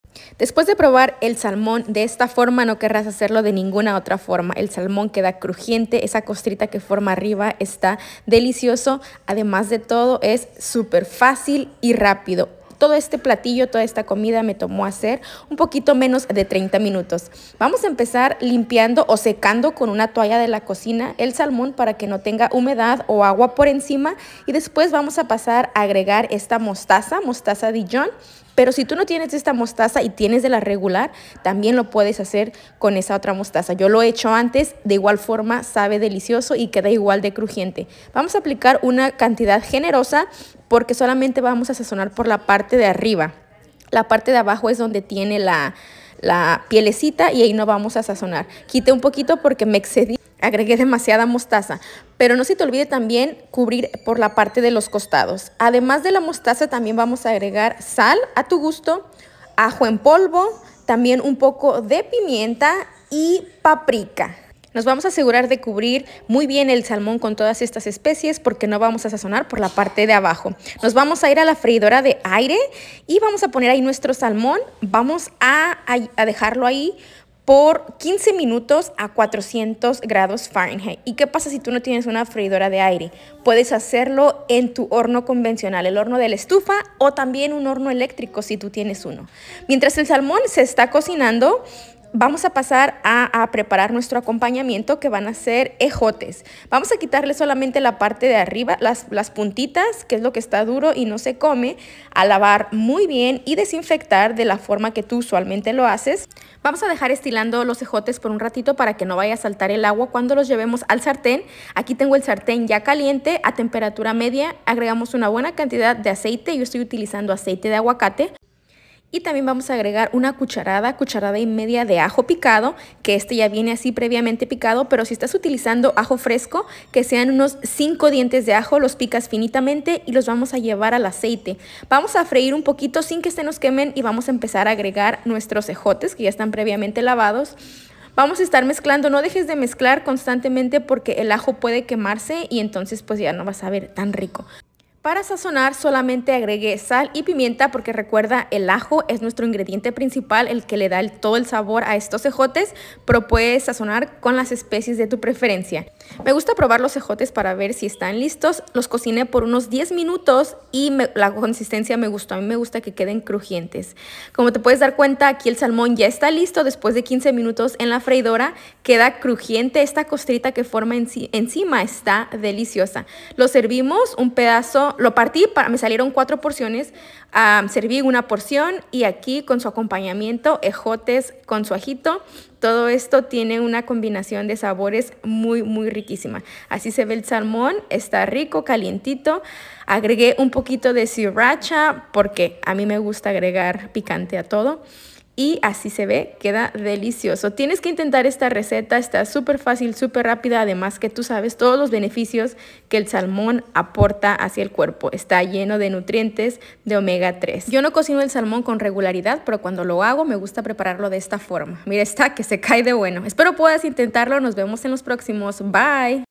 Receta De Salmon En El Air Fryer 🍱salmonenfreidoradeaire
Te comparto esta receta super facil de salmon e air fyer y ejotes como acomanamiento.
Después de preparar el salmón de esta forma, no lo querrás hacer de otra forma🍣 😋